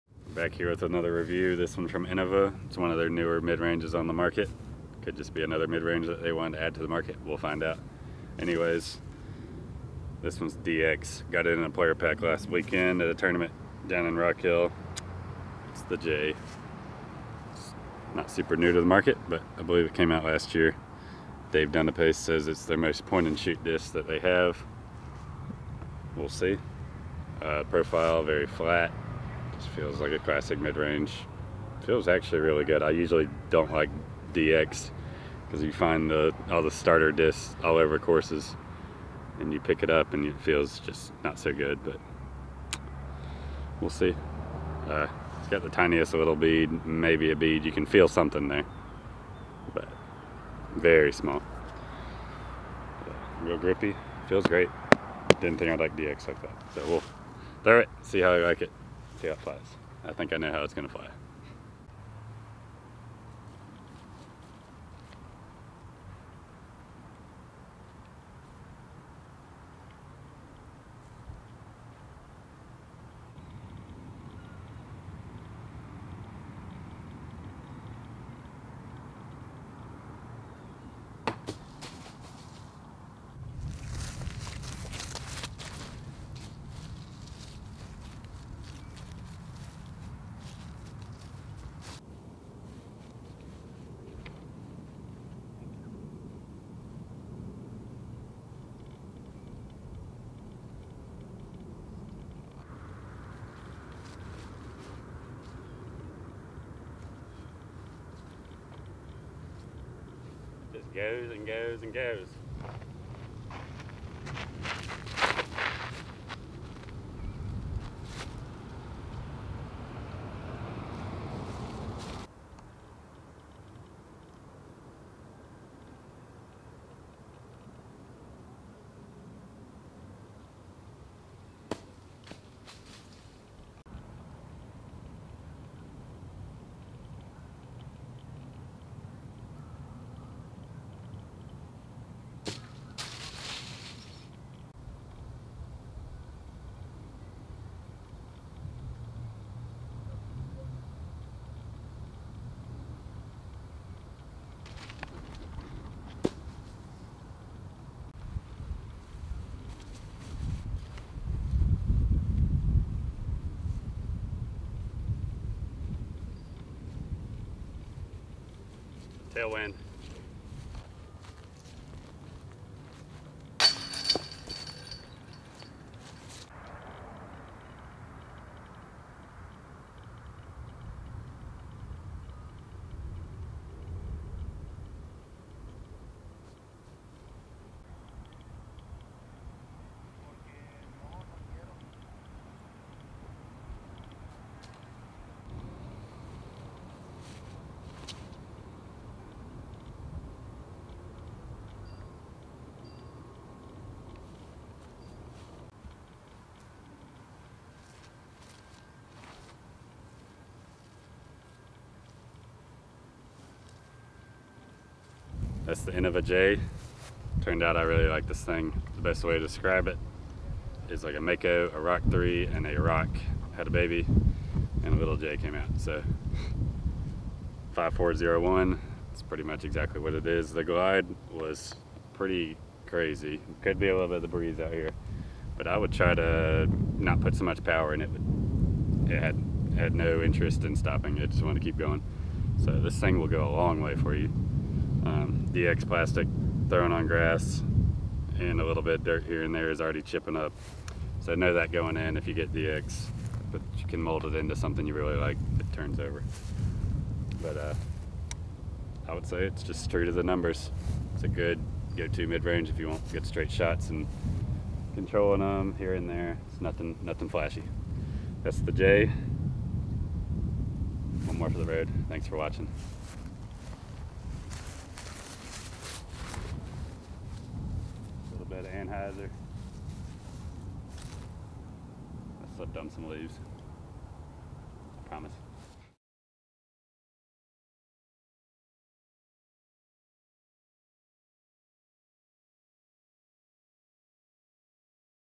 Jay from Innova - Review
Craig reviews the Jay from Innova, a straight flying midrange that was released last year. Although expectations weren't too high, meaning we didn't think it would be anything special, it was actually really nice to throw. It has tons of glide and is very controllable! It might even make the bag...

The Innova Jay is a micro-beaded Mid-Range with a true-to-release flight, and it will be a staple in bags everywhere. This all-purpose disc can handle power or be finessed. It has better torque resistance than most Mid-Ranges, but still finishes straight. It is one of Innova's most point-and-shoot discs on the market. The Jay’s low-profile design fits comfortably in your hand for a clean release every throw.